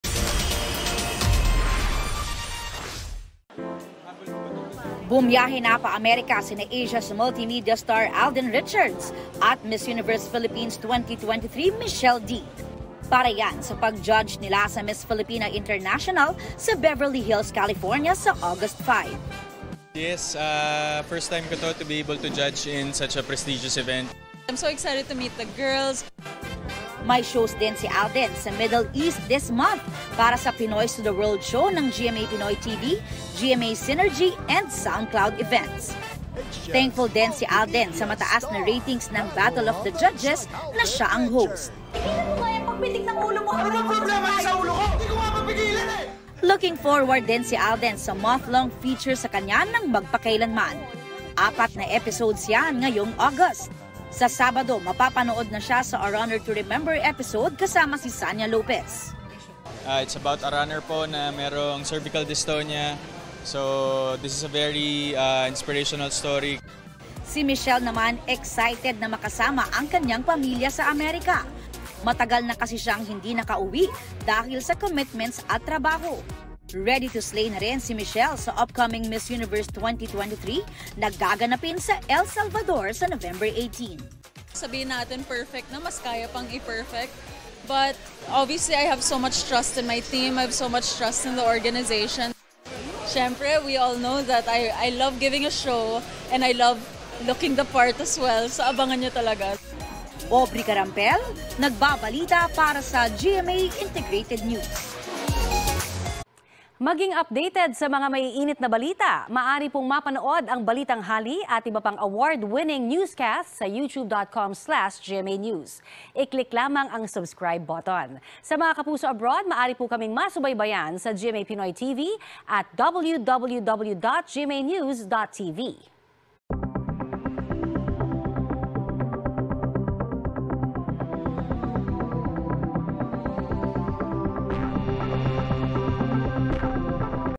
Alden Richards at Michelle Dee, judges sa Miss Filipina International... | BT
Alden Richards at Michelle Dee, judges sa Miss Filipina International pageant sa California, U.S.A.

Bumiyahe na pa-Amerika sina Asia's Multimedia Star Alden Richards at Miss Universe Philippines 2023 Michelle Dee.